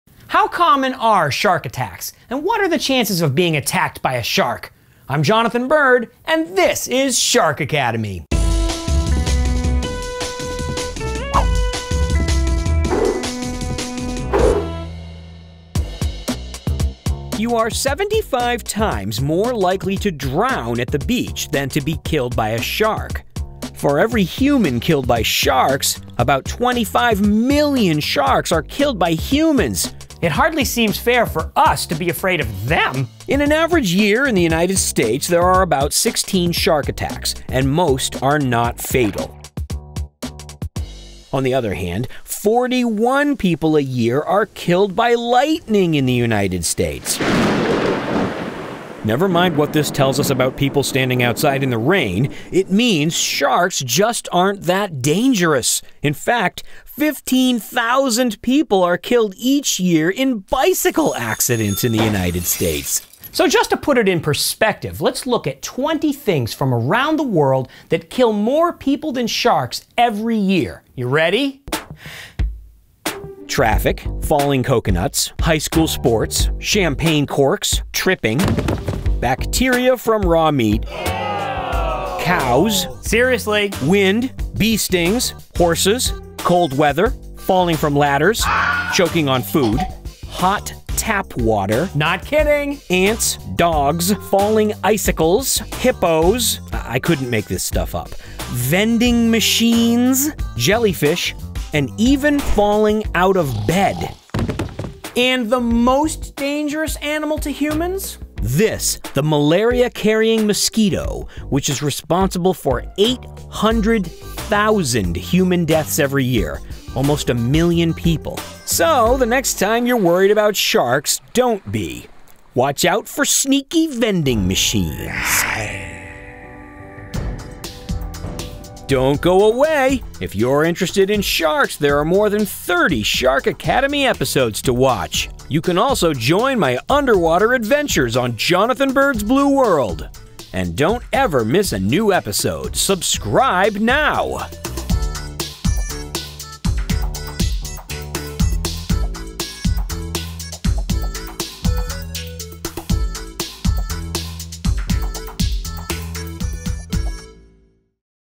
Shark Attacks | SHARK ACADEMY
In this fun and enlightening video, Jonathan Bird talks about shark attacks!  How likely are you to be attacked by a shark?  Are sharks statistically dangerous?  Find out more than you ever needed to know about shark attacks!

How common are shark attacks and what are the chances of being attacked by a shark?  I’m Jonathan Bird, and this is Shark Academy!

You are 75 times more likely to drown at the beach than to be killed by a shark.

And…for every human killed by sharks, about 25 million sharks are killed by humans.

 It  hardly seems fair for us to be afraid of them.

In an average year in the USA, there are only about 16 shark attacks, and most are not fatal.  On the other hand, 41 people a year are killed by lightning in the USA!  Nevermind what this tells us about people standing outside in the rain, it means sharks just aren’t that dangerous.  In fact, 15,000 people are killed each year in bicycle accidents in the USA.

So just to put it in perspective, let’s look at 20 things around the world that kill more people every year than sharks:  Traffic, falling coconuts, high school sports, champagne corks, tripping, choking on food, bacteria from raw meat, cows.  Seriously.  Wind, bee stings, horses, cold weather, falling from ladders, hot tap water.  Seriously., ants, dogs, falling icicles, Hippos.  I couldn’t make this stuff up.  Lightning, vending machines, jellyfish, and even falling out of bed (not kidding!)  And the most dangerous animal to humans?  This:  the malaria-carrying mosquito, which is responsible for 800,000 human deaths every year.  So next time you are worried about sharks, don’t be.  Watch out for sneaky vending machines!